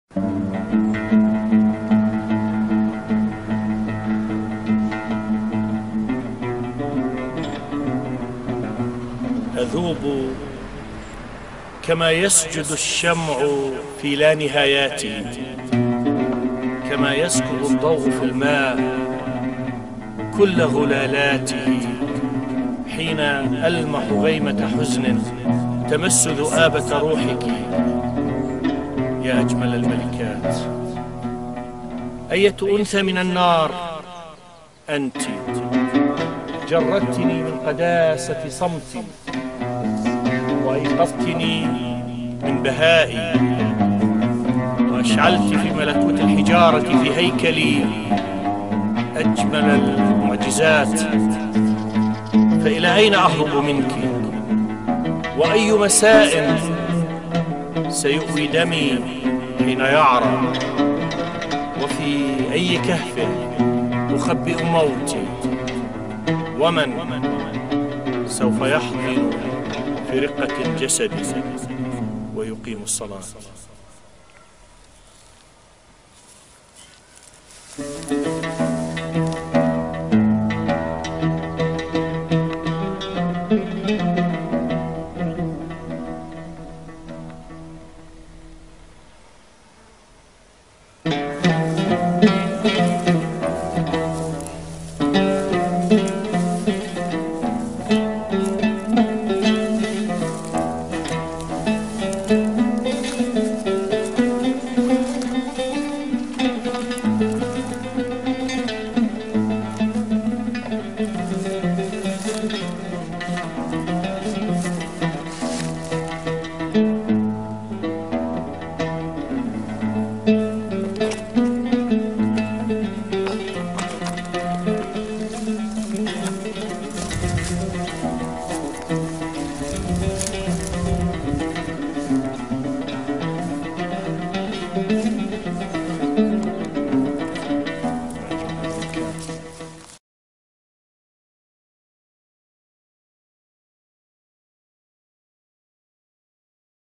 رحيل الشاعر جريس سماوي وزير الاعلام السابق
رحل الصديق الاديب والشاعر جريس سماوي رحمه الله
تصوير هاني الاردن